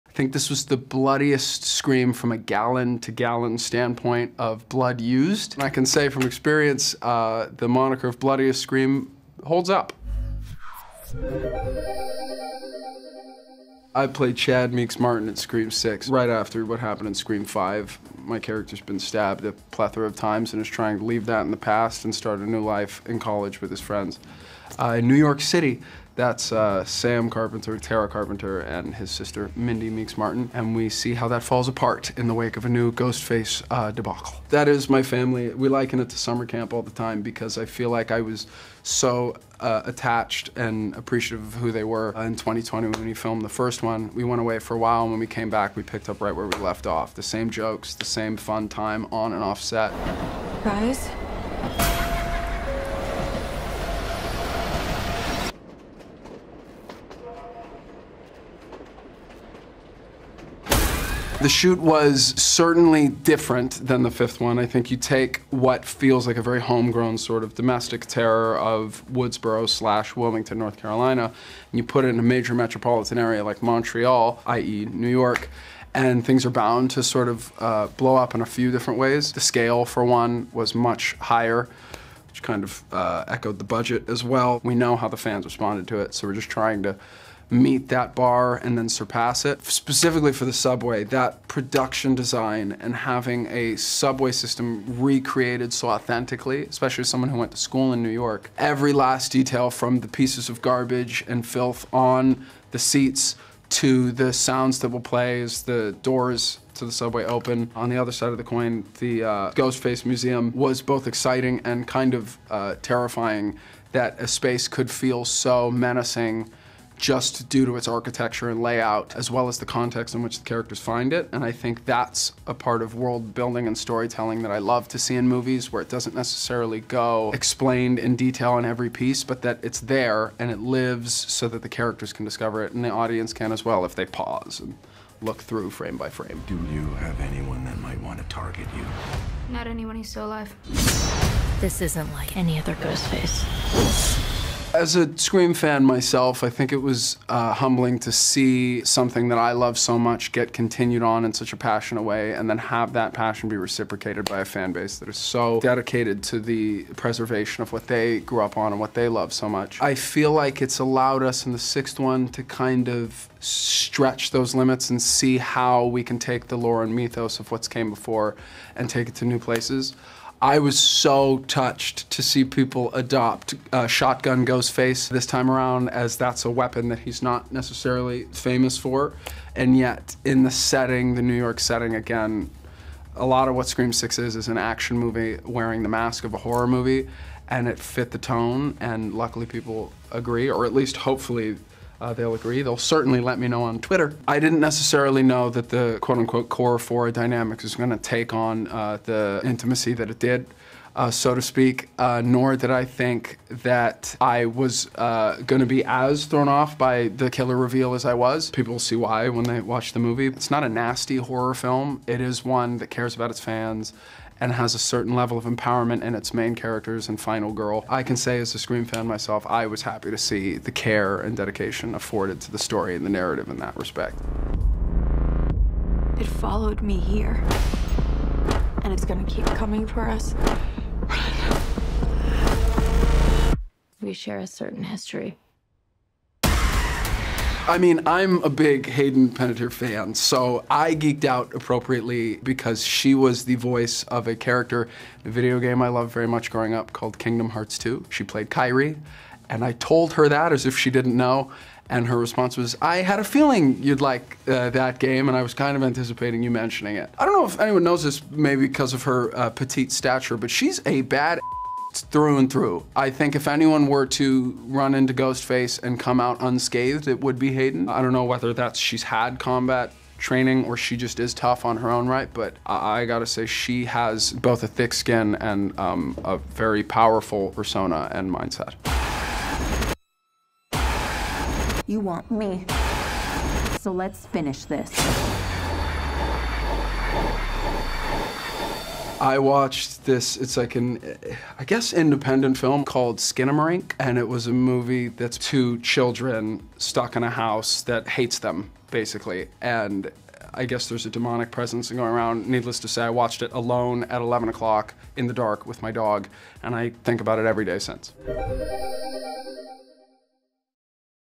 Mason Gooding on Honoring the 'Scream' Franchise | Entertainment Weekly
Mason Gooding chats with EW about how Scream VI honors the franchise's legacy.